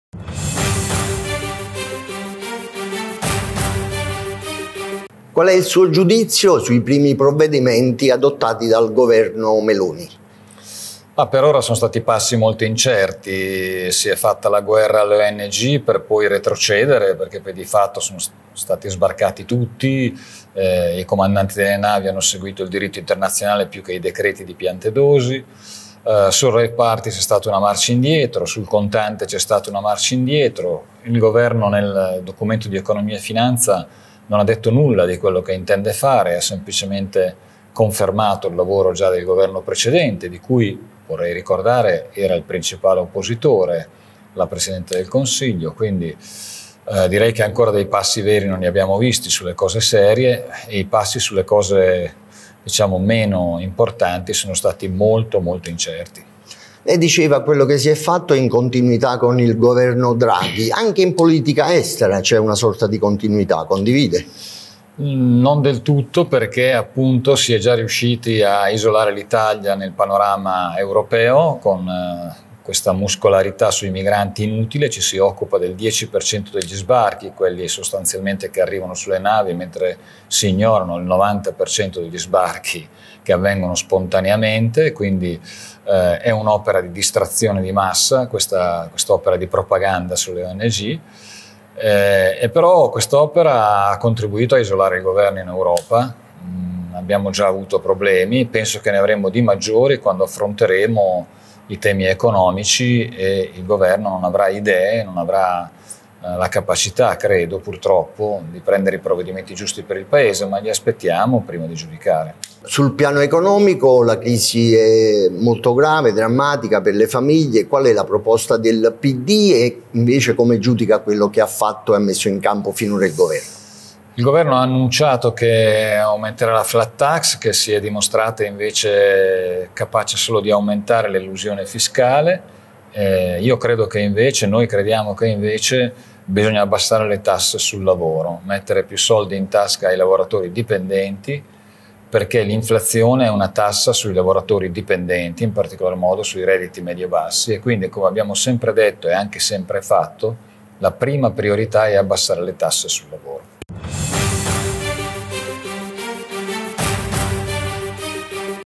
Delrio (Pd): "Priorità l'abbassamento delle tasse sul lavoro, passi del Governo molto incerti"
Rifugge dalla demagogia e va nel merito dei problemi criticando il governo Meloni sulla scorta dei primi "passi incerti" del nuovo esecutivo. Ai microfoni di Campania Notizie Graziano Delrio boccia l'azione della maggioranza di centrodestra: "Sui migranti è stata fatta un'opera di distrazione di massa. Per il resto ci sono stati solo passi indietro come sui rave party o sul tetto dei contanti